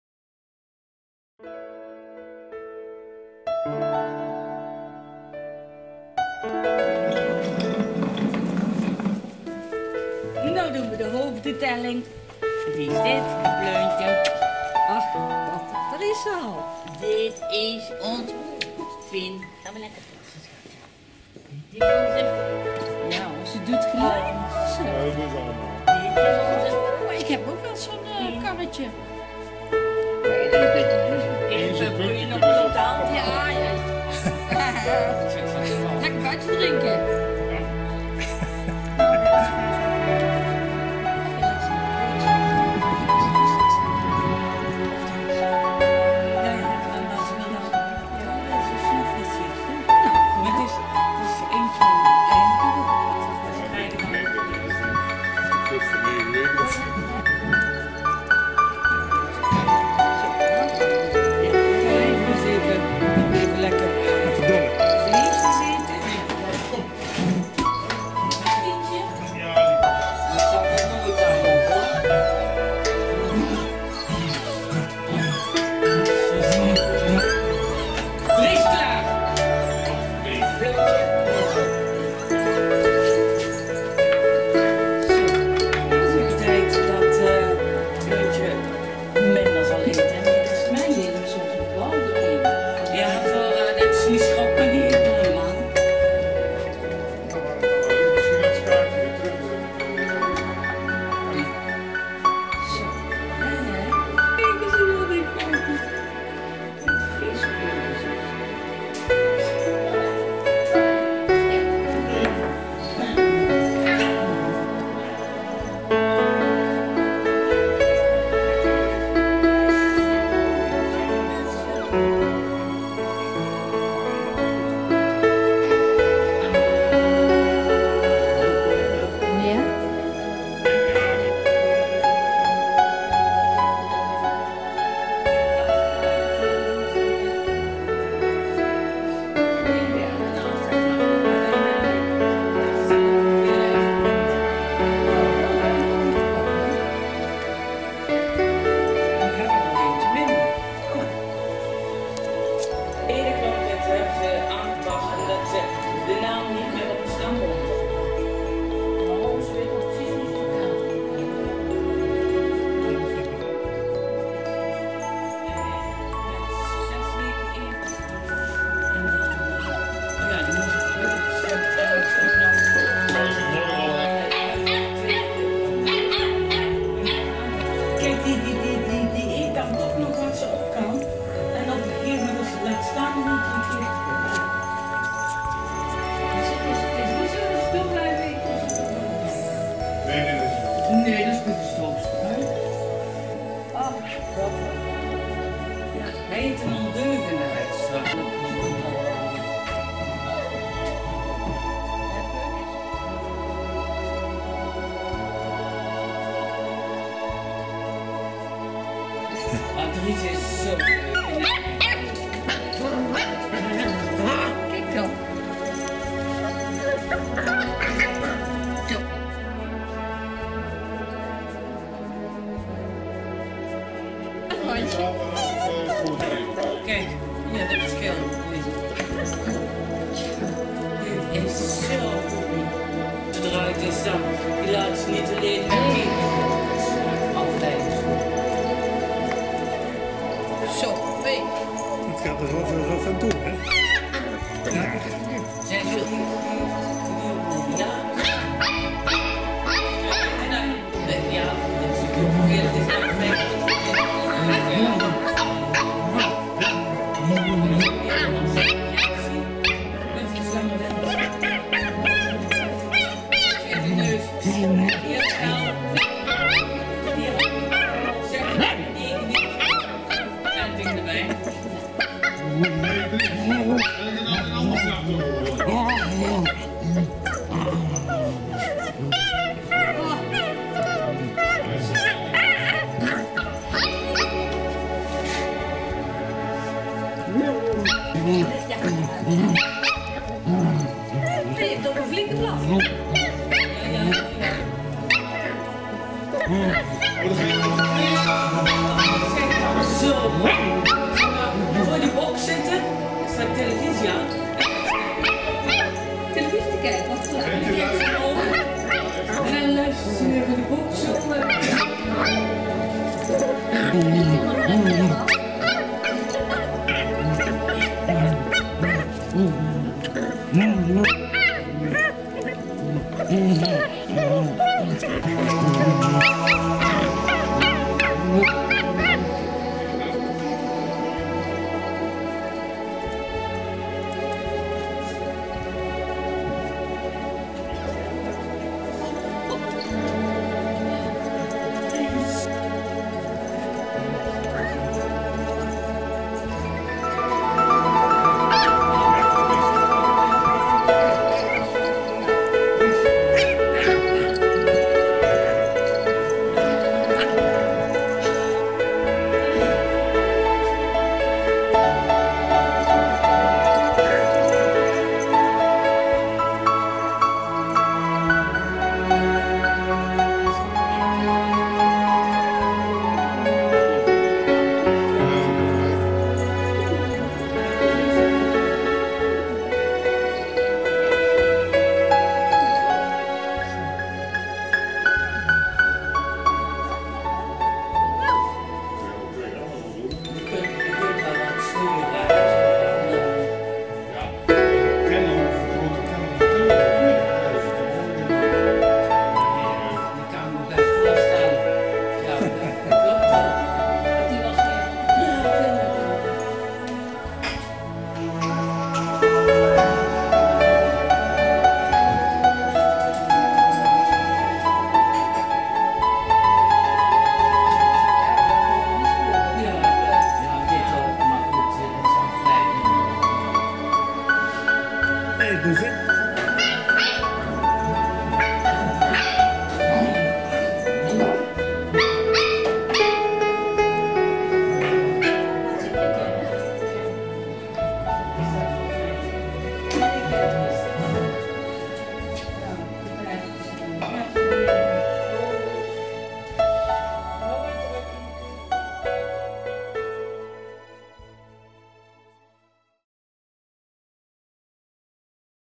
Afscheid en een laatste dolletje
Op 6 februari 2015 mochten we ons Pleuntje ophalen bij Elly en Peter Ruijgh in Cuijk. Als je de beelden bekijkt, die we maakten op de dag van het afscheid, dan vraag je je af hoe zo'n beestje zo'n heftige overgang (over)(be)leeft.